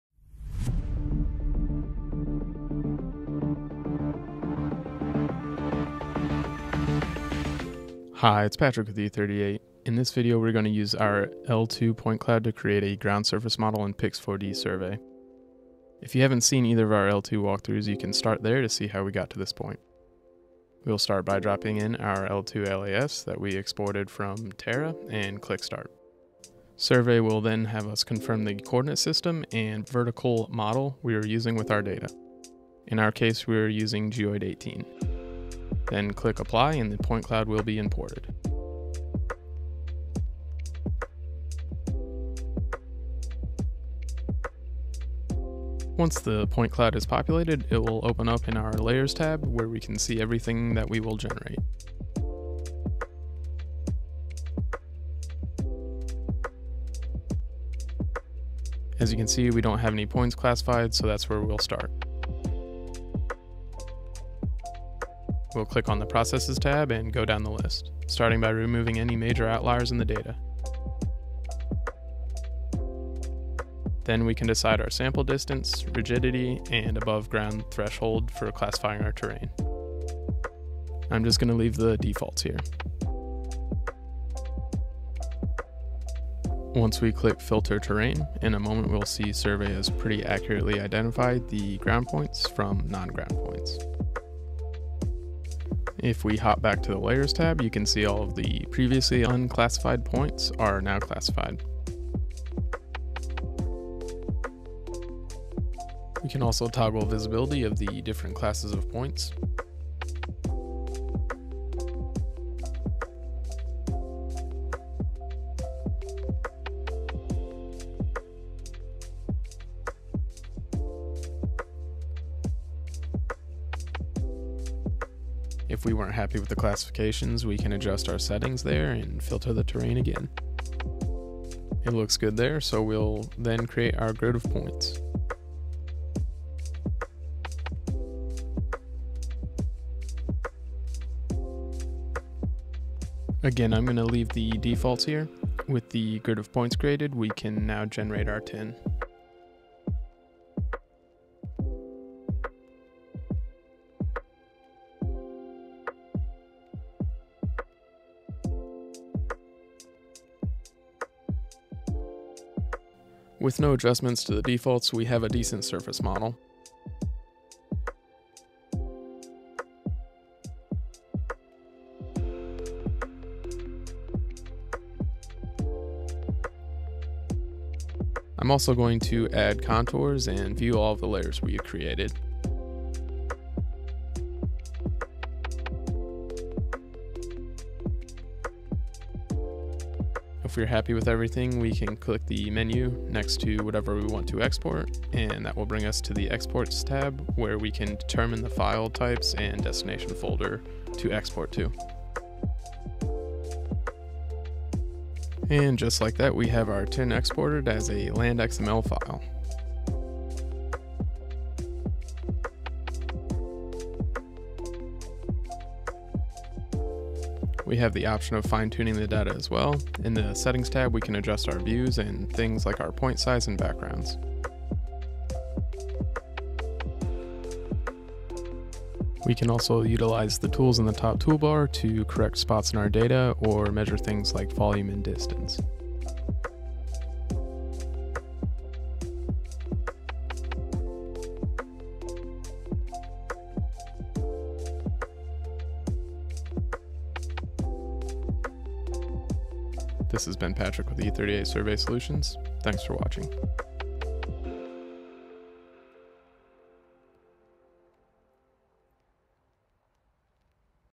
Creating a TIN Surface with the DJI Zenmuse L2 and PIX4Dsurvey
In this video we demonstrate how easy and powerful the PIX4Dsurvey software is by producing a usable landXML file while running all automation with default settings.